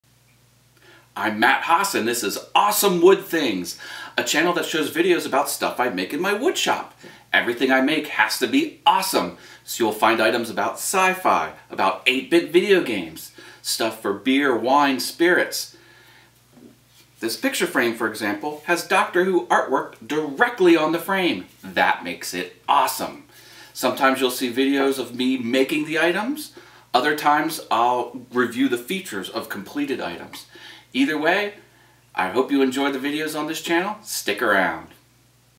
Channel Trailer
Videos about awesome things I make in my woodshop! You'll see Sci-Fi related items, 8-bit video game themed items, and lots of stuff for beer, wine & spirits.
Stick around!!